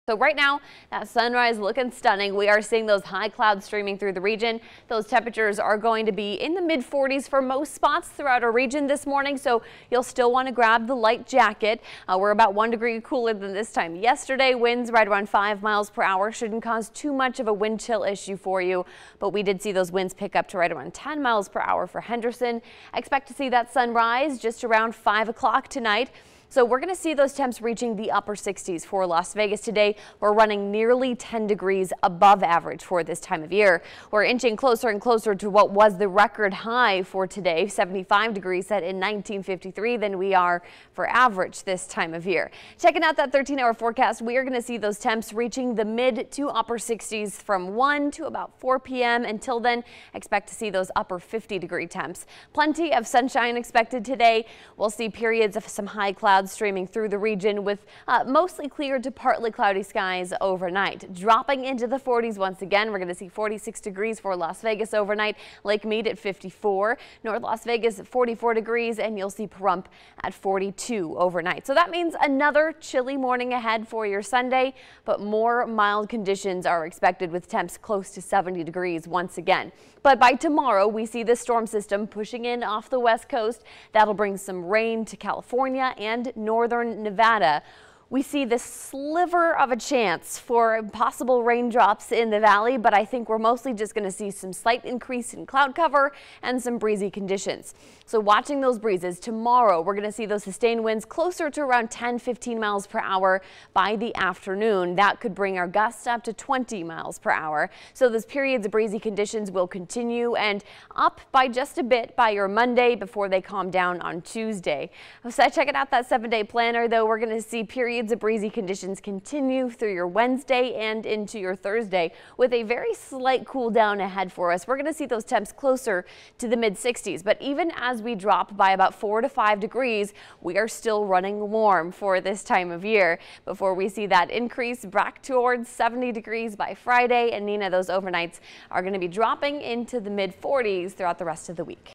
13 First Alert Las Vegas morning forecast | Jan. 25, 2020
The Jan. 25, 2020, morning weather forecast for Las Vegas.